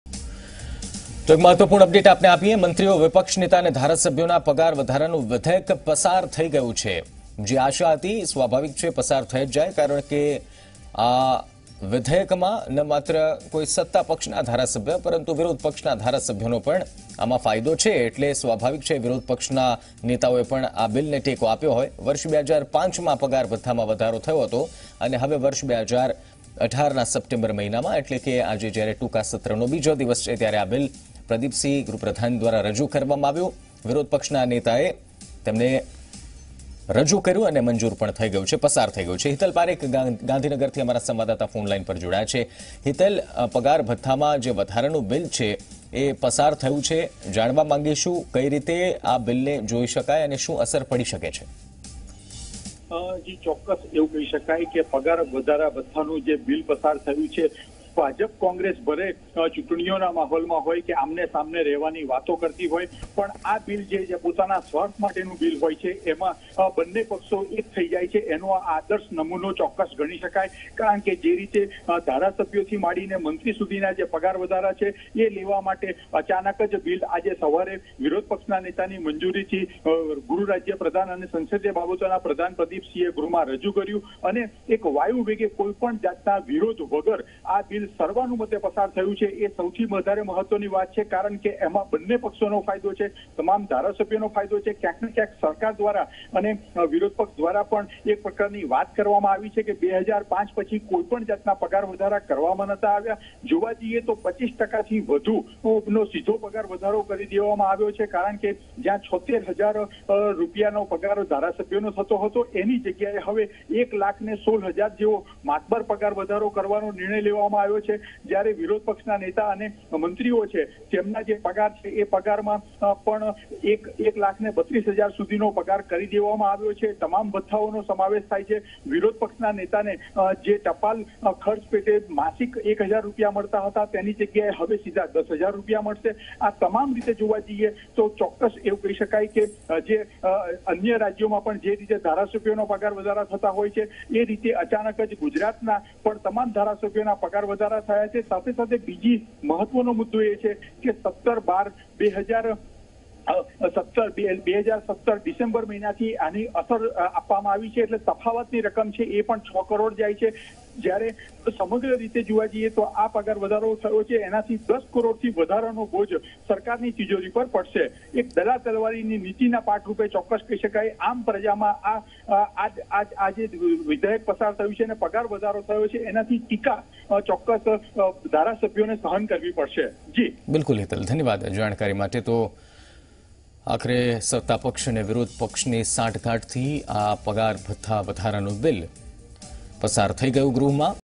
Gujarat MLAs, ministers, opposition leader to draw more salary - Zee 24 Kalak
Gujarat MLAs, ministers, opposition leaders to draw more salary